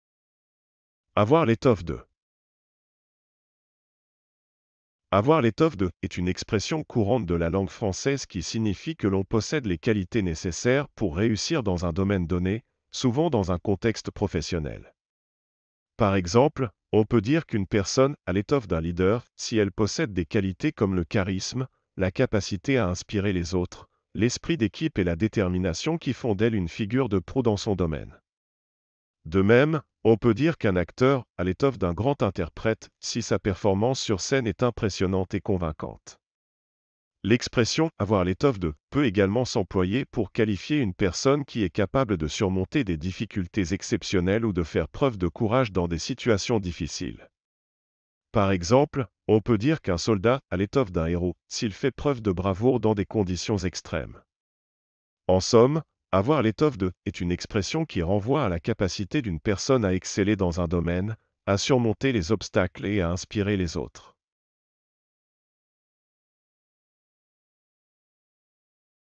Avoir l'étoffe de
"Avoir l'étoffe de" est une expression courante de la langue française qui signifie que l'on possède les qualités nécessaires pour réussir dans un domaine donné, souvent dans un contexte professionnel.
Par exemple, on peut dire qu'une personne "a l'étoffe d'un leader" si elle possède des qualités comme le charisme, la capacité à inspirer les autres, l'esprit d'équipe et la détermination qui font d'elle une figure de proue dans son domaine. De même, on peut dire qu'un acteur "a l'étoffe d'un grand interprète" si sa performance sur scène est impressionnante et convaincante.
L'expression "avoir l'étoffe de" peut également s'employer pour qualifier une personne qui est capable de surmonter des difficultés exceptionnelles ou de faire preuve de courage dans des situations difficiles. Par exemple, on peut dire qu'un soldat "a l'étoffe d'un héros" s'il fait preuve de bravoure dans des conditions extrêmes.
En somme, "avoir l'étoffe de" est une expression qui renvoie à la capacité d'une personne à exceller dans un domaine, à surmonter les obstacles et à inspirer les autres.